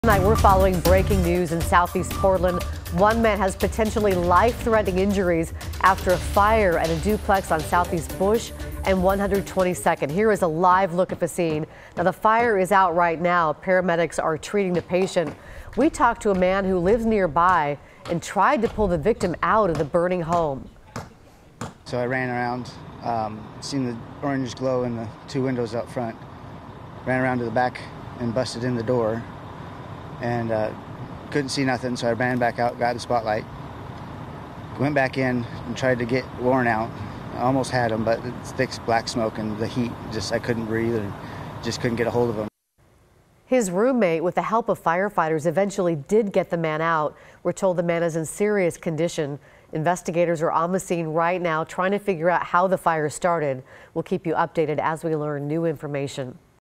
One man hurt in duplex fire
One person was seriously hurt in a duplex fire in SE Portland.